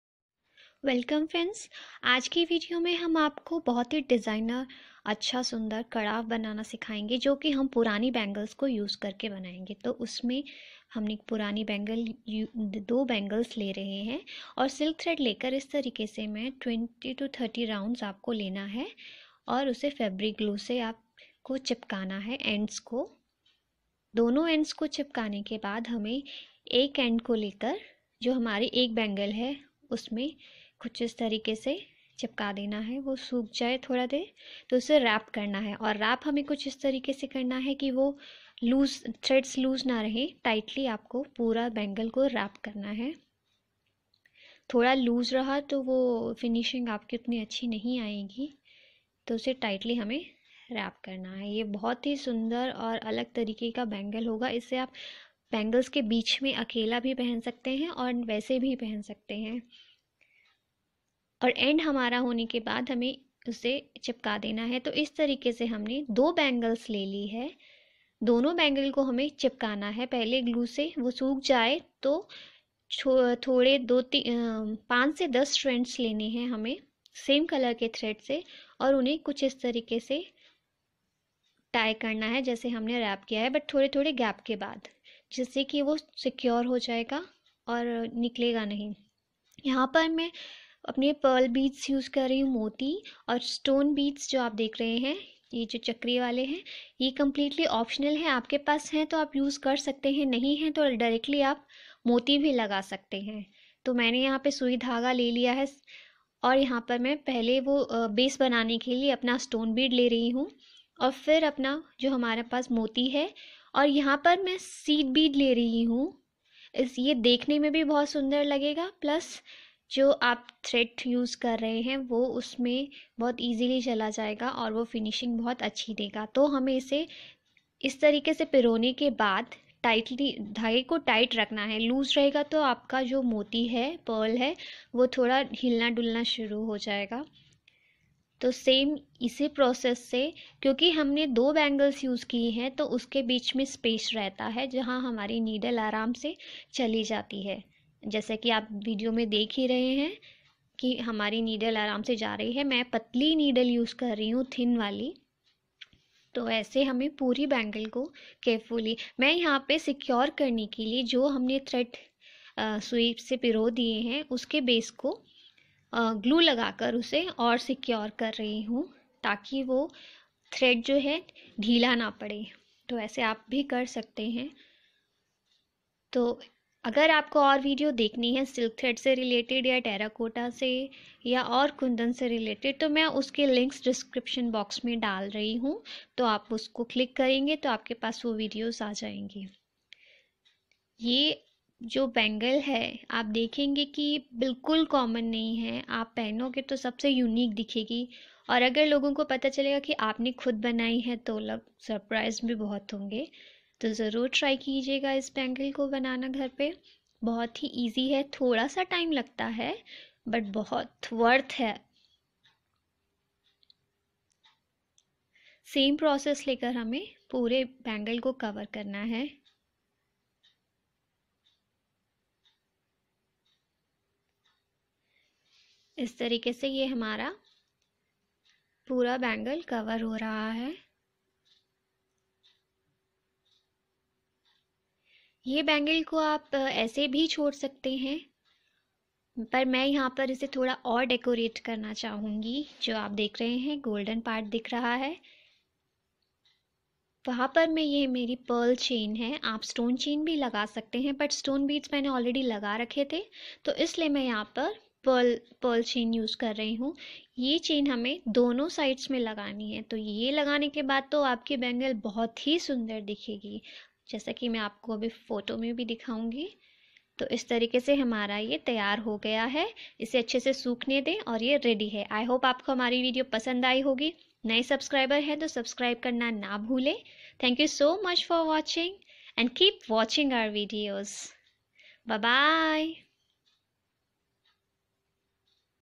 How to Make Designer Pearl Bangle From Old Bangles/Craft from waste bangles.
In this video you will see how to make Beautiful Trendy bangle making from old bangles. Keep watching our channel to get many more creative videos. Thank you

For More